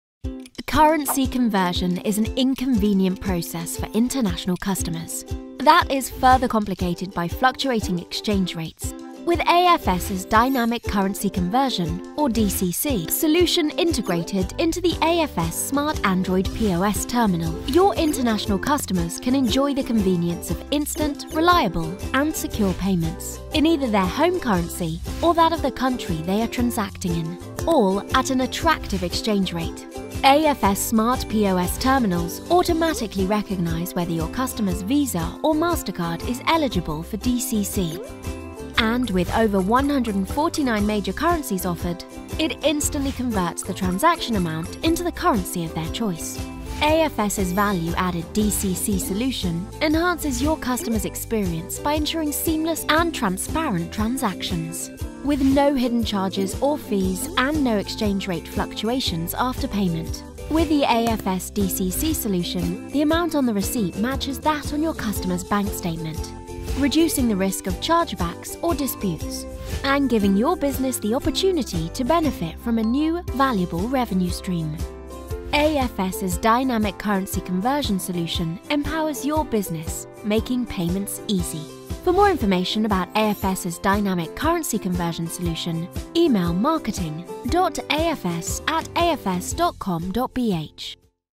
AFS DCC | 2d Explainer Video | Doodlemango
Here is our yet another project that explains the working of AFS Smart android POS terminal. Have a glimpse of our efforts to break down the complexity in explaining the features and advantages in the POS machine. Comment on your thoughts about the video.